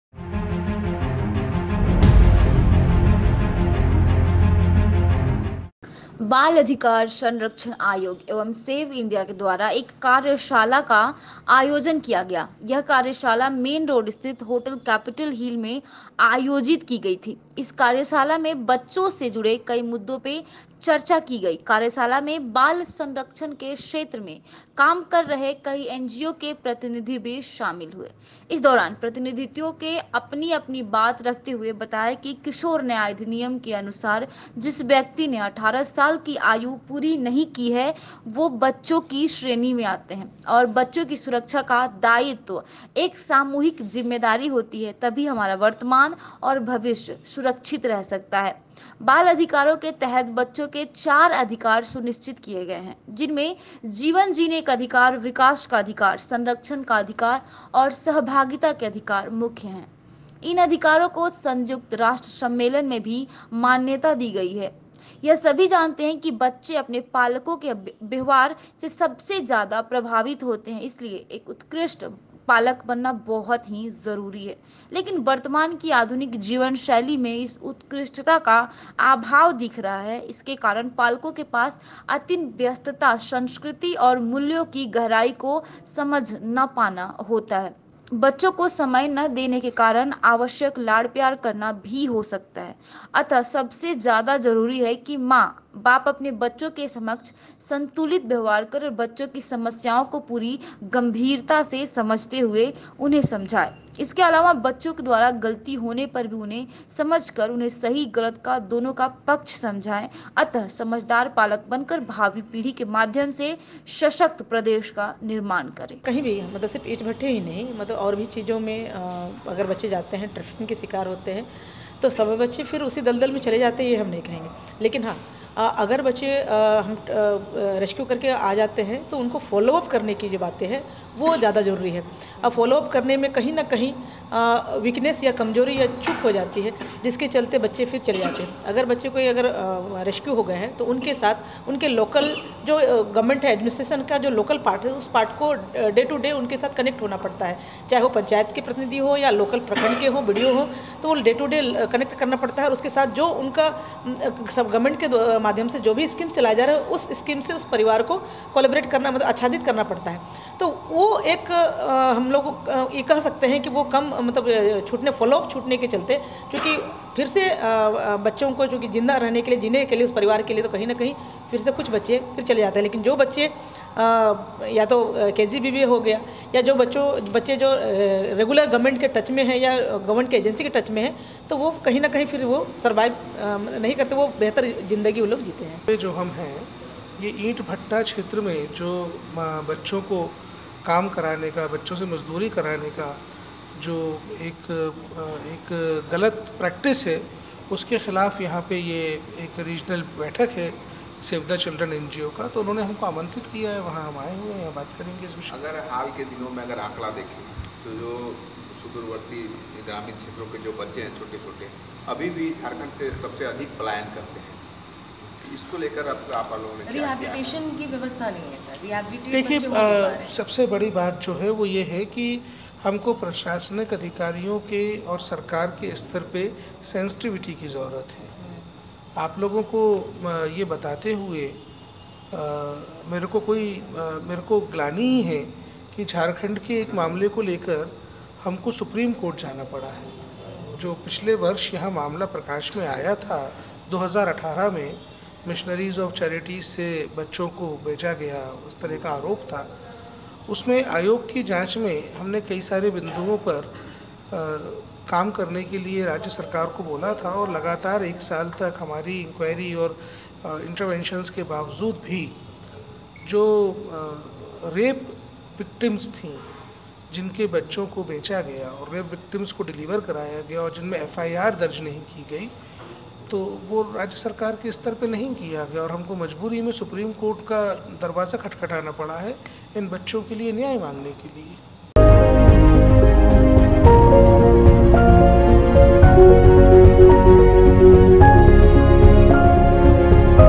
बाल अधिकार संरक्षण आयोग एवं सेव इंडिया के द्वारा एक कार्यशाला का आयोजन किया गया||P3NEWSJHARKHAND
राँची :-  बाल अधिकार संरक्षण आयोग एवं सेव इंडिया के द्वारा एक कार्यशाला का आयोजन किया गया। यह कार्यशाला मेन रोड स्थित होटल कैपिटल हिल में आयोजित की गई थी। इस कार्यशाला में बच्चों से जुड़े कई मुद्दों पर चर्चा की गई। इस कार्यशाला में बाल संरक्षण के क्षेत्र में कार्य कर रहे कई एनजीओ के प्रतिनिधि भी शामिल हुए। इस दौरान प्रतिनिधियों ने अपनी-अपनी बात रखते हुए बताया कि  किशोर न्याय अधिनियम के अनुसार जिस व्यक्ति ने 18 साल की आयु पूरी नहीं की है वो बच्चो की श्रेणी में आते है और बच्चों की सुरक्षा का दायित्व एक सामुहिक जिम्मेदारी होती है तभी हमारा वर्तमान और भविष्य सुरक्षित रह सकता है। बाल अधिकारो के तहत बच्चों के 4 अधिकार सुनिश्चित किए गए है जिनमें जीवन जीने का अधिकार, विकास का अधिकार, संरक्षण का अधिकार और सहभागिता के अधिकार मुख्य है। इन अधिकारो को संयुक्त राष्ट्र सम्मेलन में भी मान्यता दी गई है। यह सभी जानते है कि बच्चे अपने पालको के व्यवहार से सबसे ज्यादा प्रभावित होते है इसलिए एक उत्कृष्ट पालक बनना बहुत जरुरी है। लेकिन वर्तमान की आधुनिक जीवनशैली में इस उत्कृष्टता का अभाव दिख रहा है, इसके कारण पालको के पास अति व्यस्तता, संस्कृति और मूल्यो की गहराई की समझ ना होना, बच्चो को समय न देने के कारण अनावश्यक लाड़ प्यार करना हो सकता है। अतः सबसे ज्यादा जरुरी है कि माँ-बाप अपने बच्चो के समक्ष संतुलित व्यवहार करे और बच्चो की समस्याओं को पूरी गंभीरता से समझते हुए उन्हें समझाए। इसके अलावा बच्चों के द्वारा कोई गलती होने पर उन्हें पहले समझकर उन्हें सही और गलत दोनो का पक्ष समझाये। अतः समझदार पालक बनकर भावी पीढ़ी के माध्यम से सशक्त प्रदेश का निर्माण करे।
इस कार्यशाला झारखंड से पश्चिम बंगाल जाने वाले बच्चों को वहां के स्कूलों में  होने वाली भाषाई दिक्कतों को ध्यान में रखकर किया गया. कार्यक्रम के मुख्य अतिथि के तौर पर एनसीपीसीआर के चेयरपर्सन प्रियांक कानूंगू भी कार्यशाला में शामिल हुए.